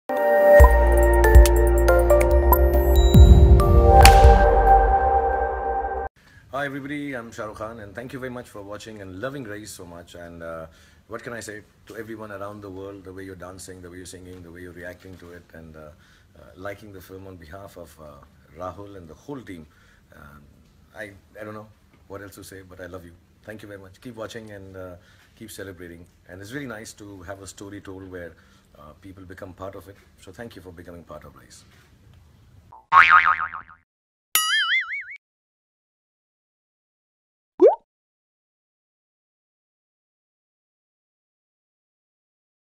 Shahrukh Khan Special Thanks To All His Fans After Watching Raees
Watch Shahrukh Khan Special Thanks To All His Fans After Watching Raees